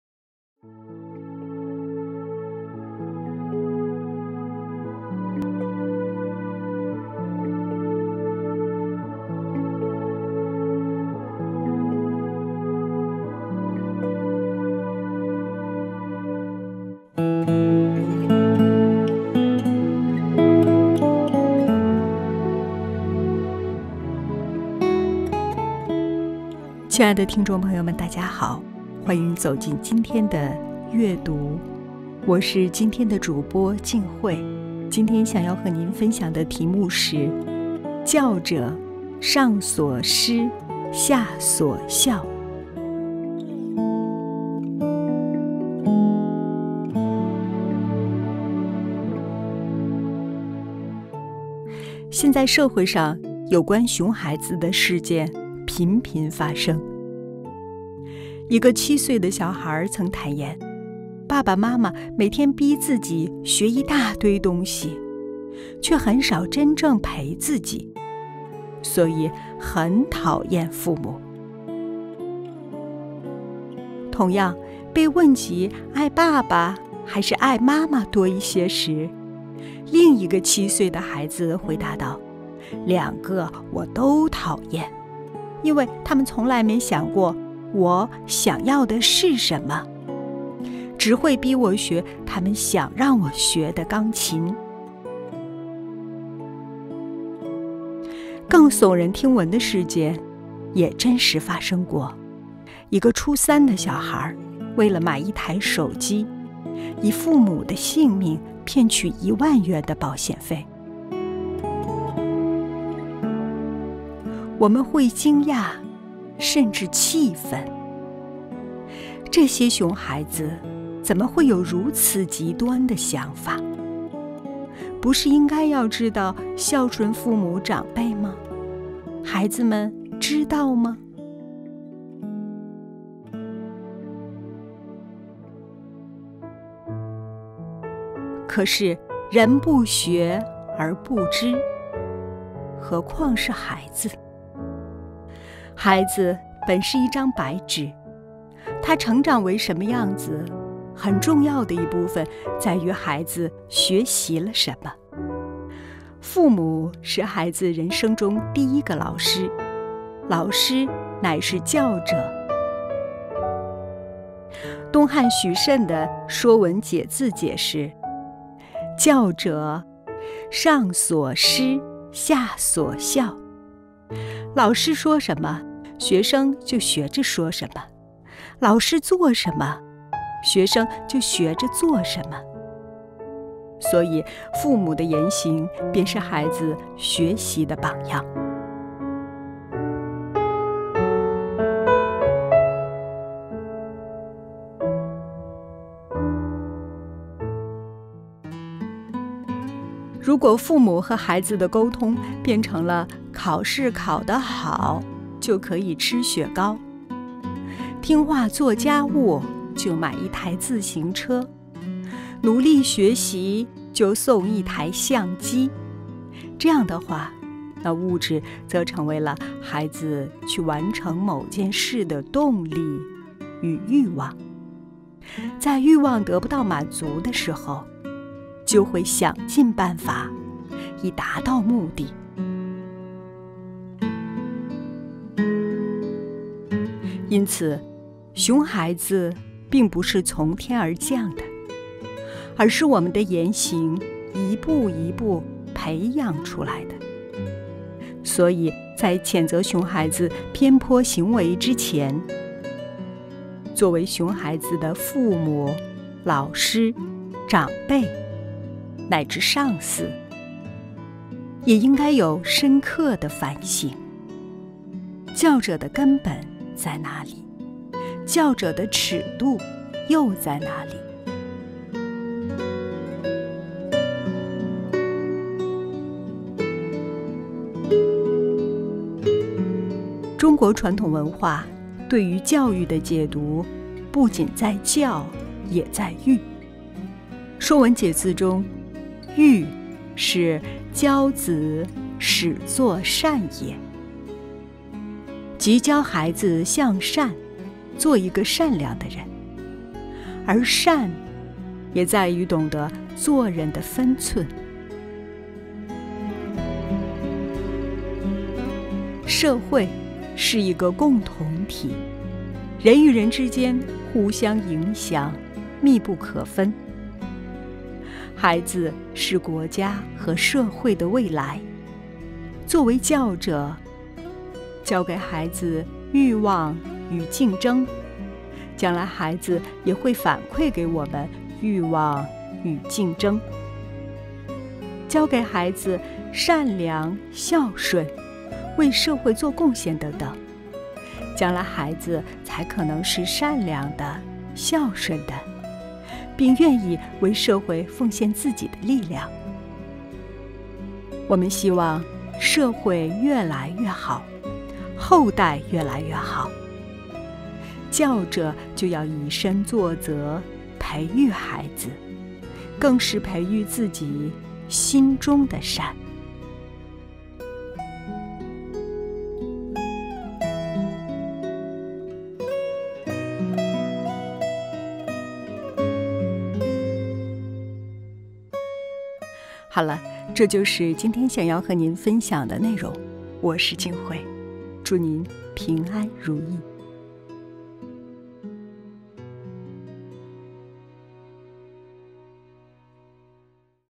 悅讀｜教者，上所施，下所效
摘要：「熊」孩子，哪裡來？

社會上有關「熊」孩子的事件頻頻發生。

一個七歲的小孩曾坦言：爸爸媽媽每天逼自己學一大堆東西，卻很少真正陪自己，所以很討厭父母。

同樣，當被問及愛爸爸還是愛媽媽多一些時，另一個七歲的孩子回答道：兩個我都討厭，因為他們從來沒想過我想要的是什麼，只會逼我學他們想讓我學的鋼琴。

更聳人聽聞的事件也真實發生過。

一個初三的小孩為了買一台手機，以父母的性命騙取一萬元的保險費。

我們會驚訝，甚至氣憤，這些「熊」孩子怎麼會有如此極端的想法？不是應該要知道孝順父母、長輩嗎？

孩子們知道嗎？

可是，人不學而不知，何況是孩子。

孩子本是一張白紙，他成長為什麼樣子，很重要的一部分在於孩子學習了什麼。

父母是孩子人生中第一個老師。

老師乃是教者。東漢許慎的《說文解字》解釋，「教者，上所施，下所效」。老師說什麼，學生就學著說什麼；老師做什麼，學生就學著做什麼。所以父母的言行便是孩子學習的榜樣。

如果父母和孩子的溝通變成了考試考得好，吃雪糕；聽話做家務，就買一台自行車；努力學習，就送一台相機。這樣的話，那物質則成為了孩子去完成某件事的動力與欲望，在欲望得不到滿足的時候，就會想盡辦法，以達到目的。

因此，「熊」孩子並不是從天而降的，而是我們的言行一步一步培養出來的。

所以，在譴責「熊」孩子偏頗行為之前，做為「熊」孩子的父母、老師、長輩乃至於上司，也應該有深刻的反省。

教者的根本在哪裡？教者的尺度又在哪裡？

中國傳統文化對於教育的解讀，不僅在「教」，也在「育」。《說文解字》中，「育」是「養子使作善也」，即教孩子向善，做一個善良的人，而「善」也在於懂得做人的分寸。

社會是一個共同體，人與人之間相互影響，密不可分。孩子是國家和社會的未來，做為「教者」，教給孩子欲望與競爭，將來孩子也會反饋給我們欲望與競爭；教給孩子善良、孝順，為社會做貢獻等等，將來孩子才可能是善良的、孝順的，並願意為社會奉獻自己的力量。

我們希望社會愈來愈好、後代愈來愈好，「教者」就要以身作則，培育孩子，更是培育自己心中的善。